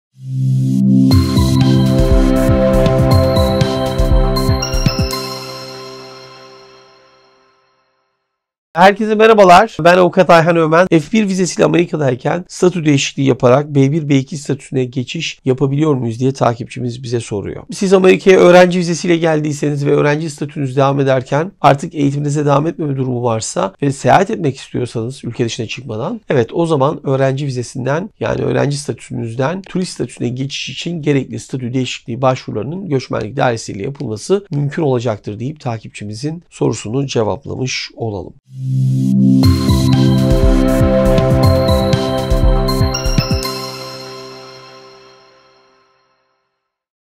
F1 ÖĞRENCİ VİZESİNİ B1/B2 TURİST VİZESİNE ÇEVİREBİLİR MİYİM ?
Soru: F1 vizesiyle Amerika’dayken statü değişikliği yaparak B1/B2 statüsüne geçiş yapabiliyor muyuz ?